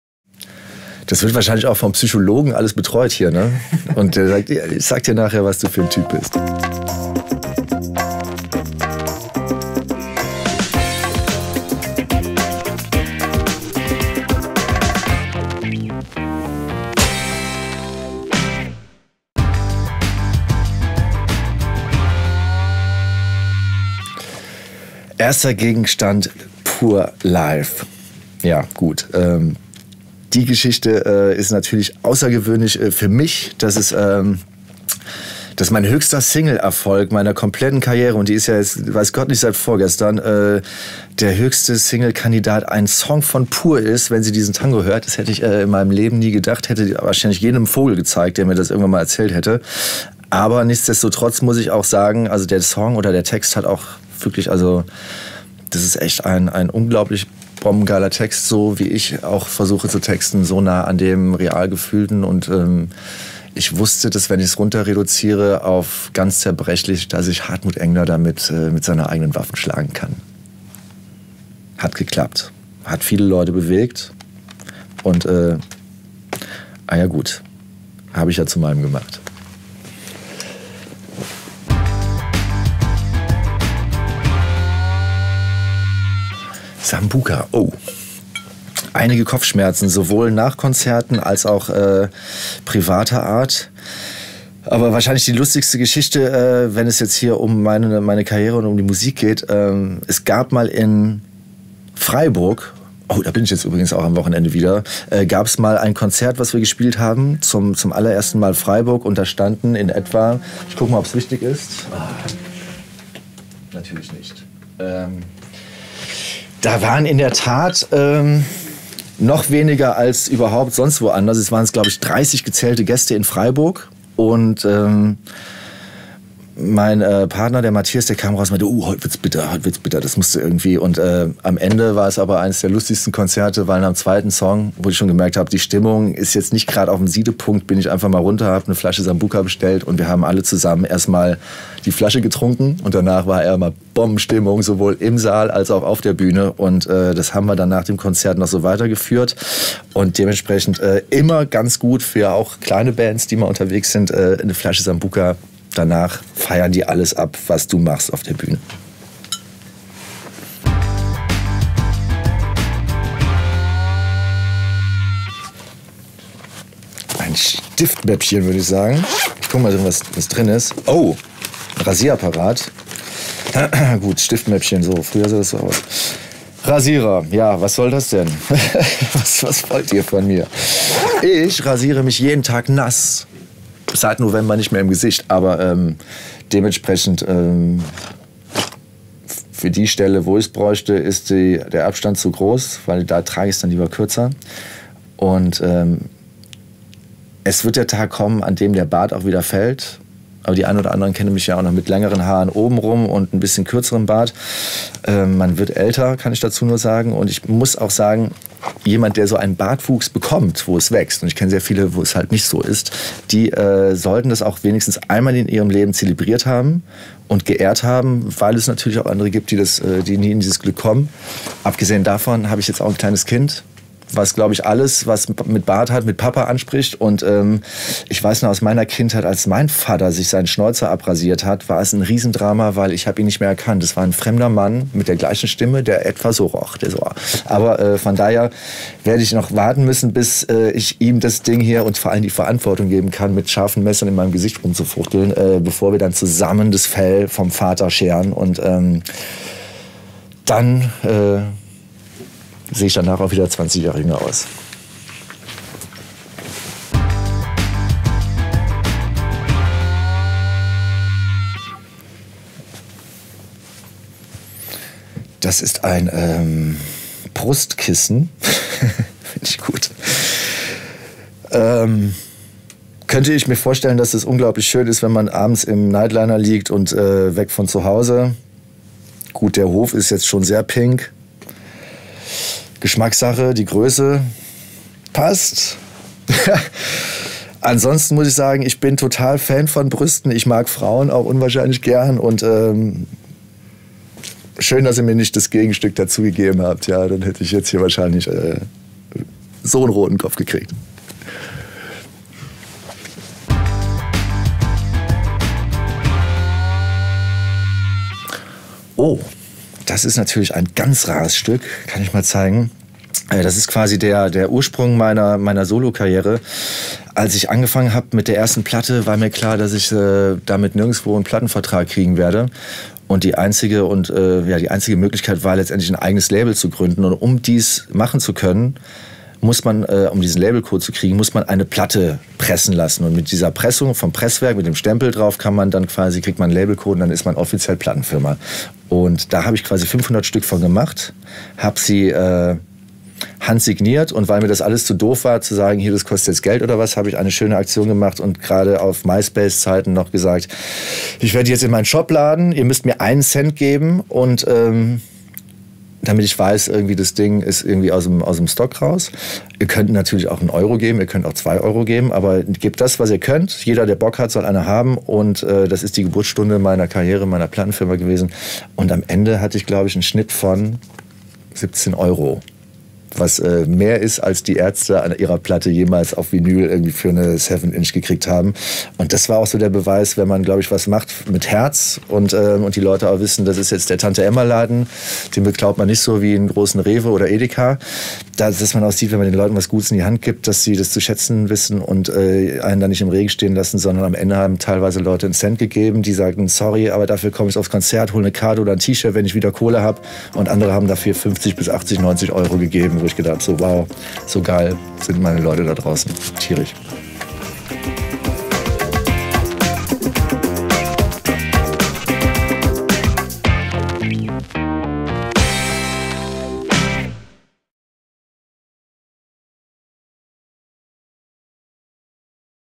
Fünf für Daniel Wirtz - das Interview ohne Fragen
Immer wenn ein Popstar, Comedian, Schauspieler oder Politiker bei den NRW-Lokalradios zu Besuch ist, stellt er sich auch dem besonderen Video-Interview „Fünf für". Dabei wird keine einzige Frage gestellt, sondern dem Gast einfach fünf Dinge in die Hand gedrückt, zu denen er das erzählt, was ihm als Erstes einfällt. Keine Standardantworten, keine Promotionaussagen - sondern ganz persönliche Geschichten - das ist „Fünf für" und gibt es auch bei Facebook